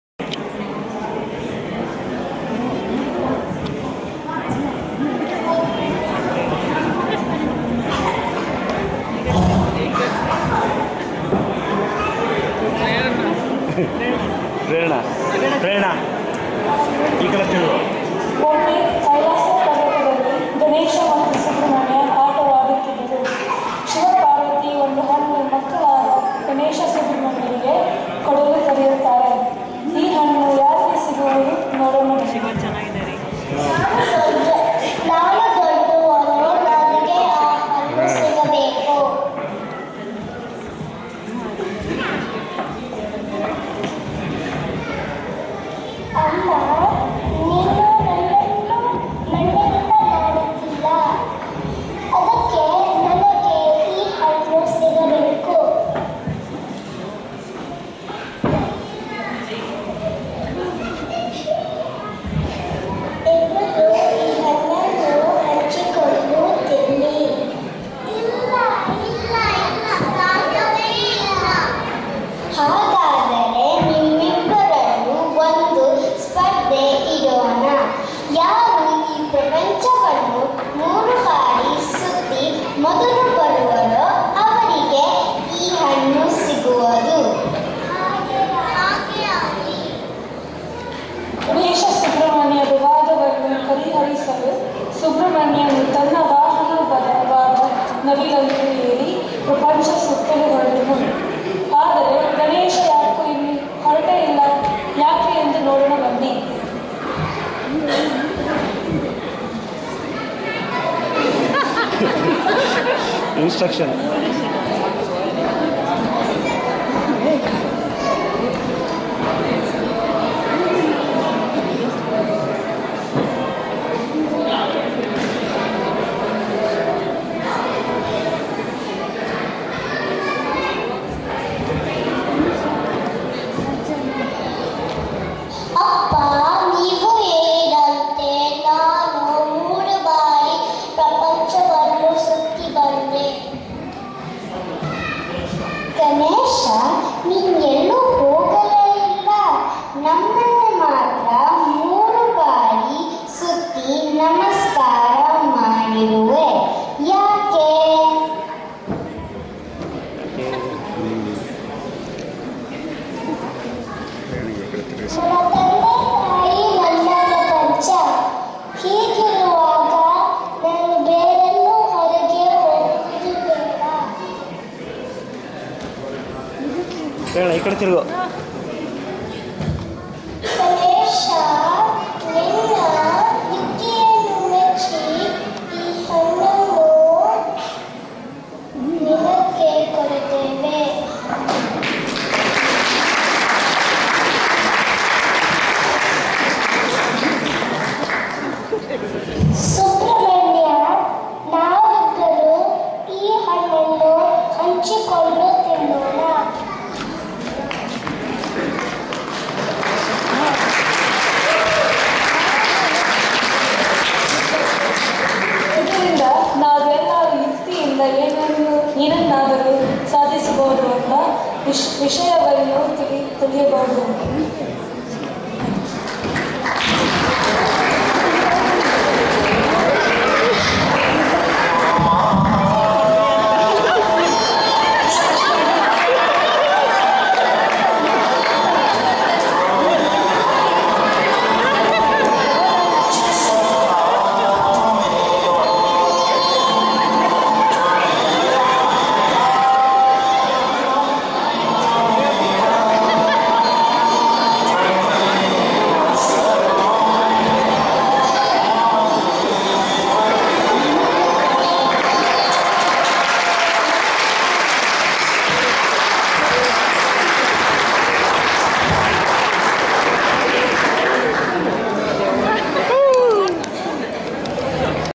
Kannada Short Skit by Brisbane Kinnara Balaga @Ganesha Festival 2013 KSQ Brisbane Australia
Performed by: Amala, Amita, Anagha, Pranavi, Prerana & Shreya
Narrated by: Rachana Nagesh
Choreographed by: Asha, Archana, Shwetha, Shilpa & Rachana